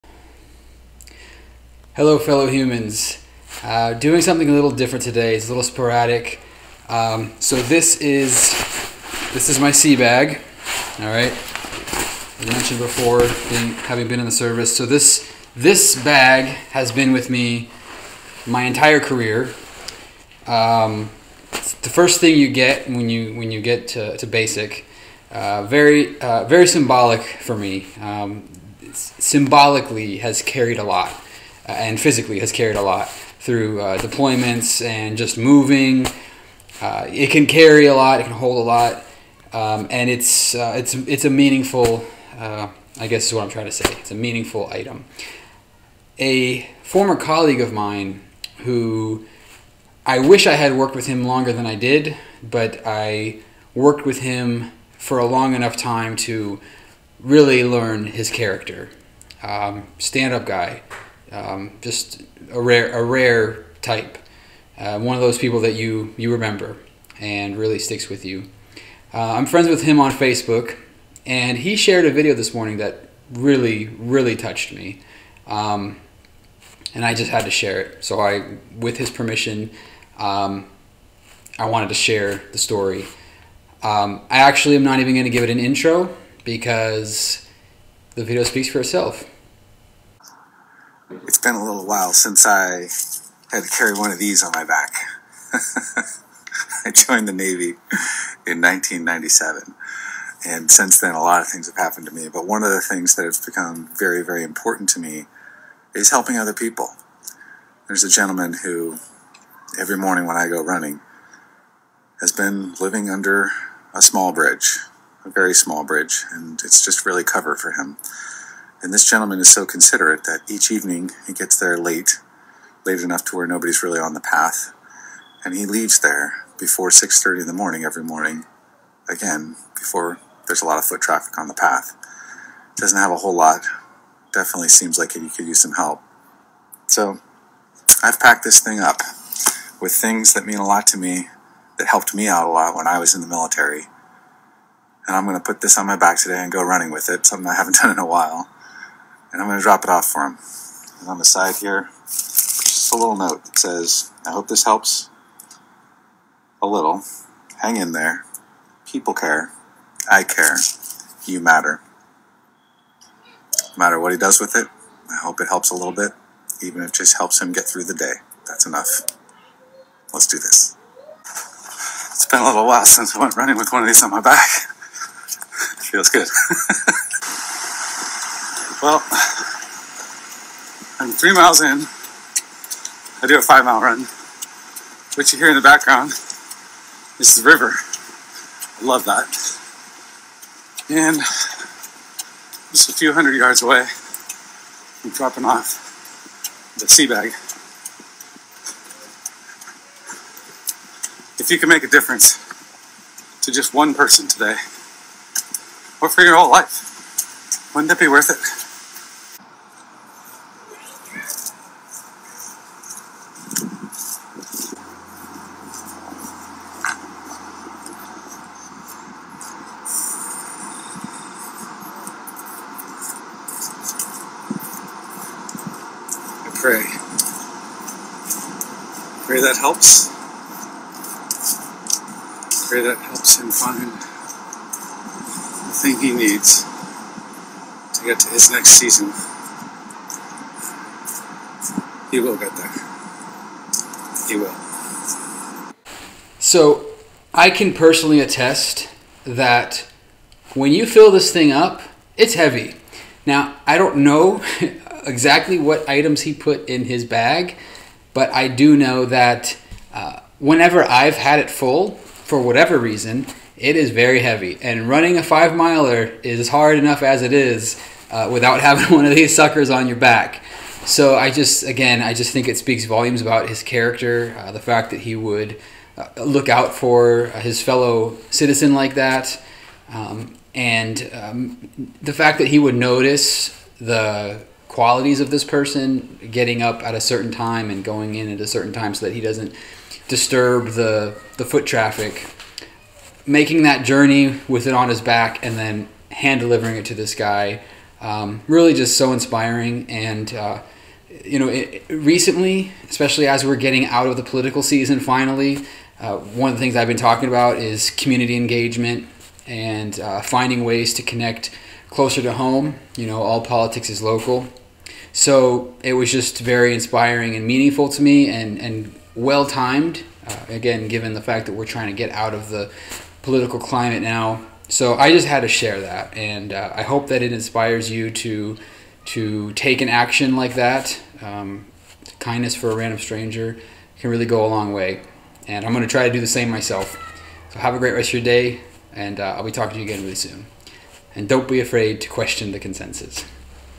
Words on Veteran's Day 2020--Service to Others Doesn't End With Military Service
Today I'd like to share a heartwarming and inspiring story from an old friend and colleague of mine.  Rule 303: If you have the means, you have the responsibility.  Will Bowden lives this rule every day.

Background Image by tookapic from Pixabay